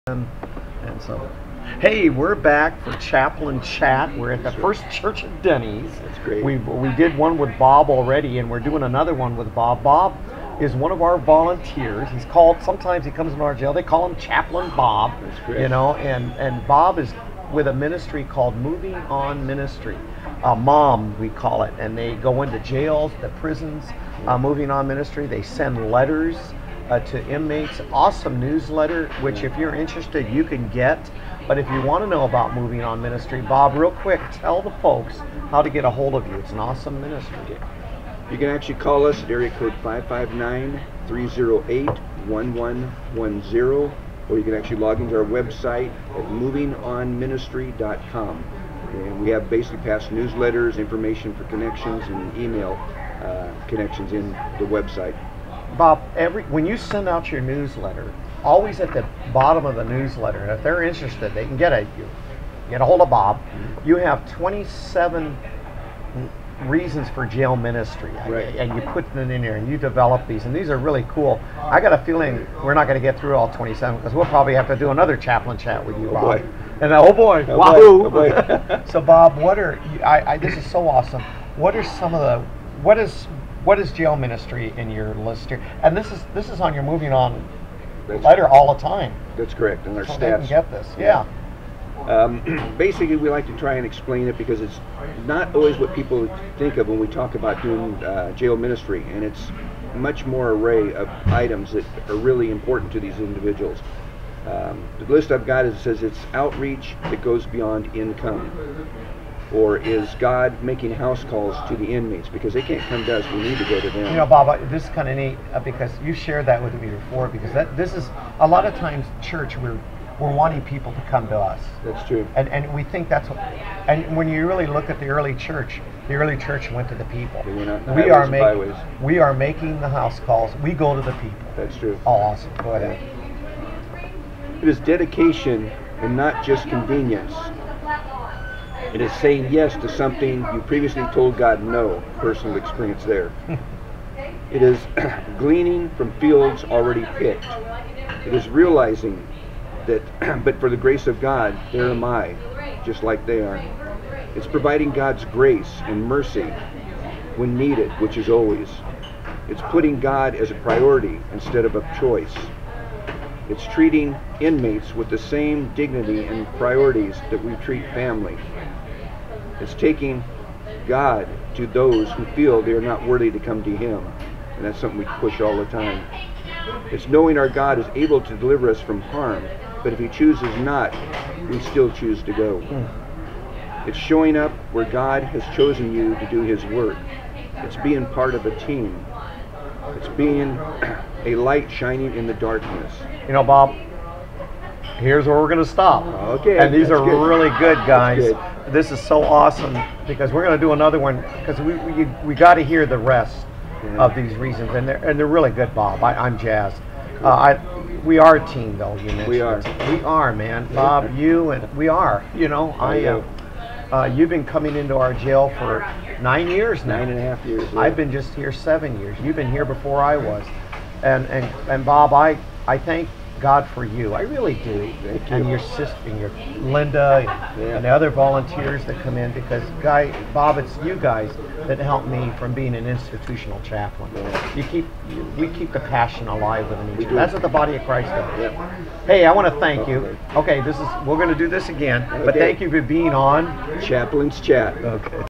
Chaplain Chat with Bob part 2.avi
Chaplain Chat at Denny's with Bob, who is one of our volunteers and also founder of MOVING ON MINISTRY...or as we would call it M.O.M.